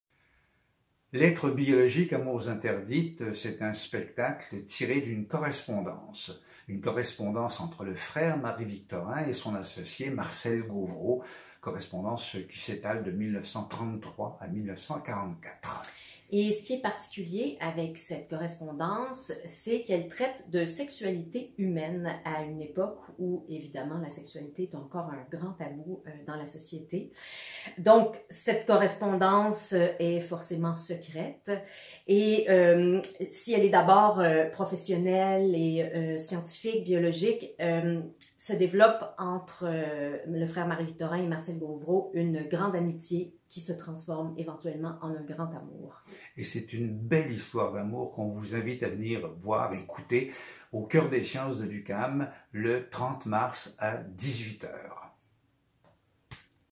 Invitation - Une belle histoire d'amour au Coeur des sciences! - 30 mars 2023, 18h
Lettres biologiques, amours interdites, un spectacle qui met en scène Yanick Villedieu et Marika Lhoumeau dans les rôles du frère Marie-Victorin et Marcelle Gauvreau.

Le récit d'une relation épistolaire secrète sur la sexualité humaine qui se transforme en une magnifique histoire d'amour.

Avec Yanick Villedieu, journaliste, écrivain et conférencier et Marika Lhoumeau, actrice, écrivaine, dramaturge et metteuse en scène québécoise.

Textes : Frère Marie-Victorin, Lettres biologiques (Éditions du Boréal, 2018) / Marcelle Gauvreau, Lettres au frère Marie-Victorin (Éditions du Boréal, 2019)

Choix et montage des textes : Marika Lhoumeau et Yanick Villedieu

Mise en lecture : Marika Lhoumeau

Avec Marika Lhoumeau et Yanick Villedieu

Voix – Lecteur de nouvelles : Raymond Archambault

Conception sonore : Éric Forget

Accessoires et costumes : Pierre-Luc Boudreau

Photo : Marie-Andreé Lemire

Production FIL 2020 : spectacle créé le 19 septembre 2020 au théâtre Outremont, dans le cadre du 26e Festival international de la littérature (FIL)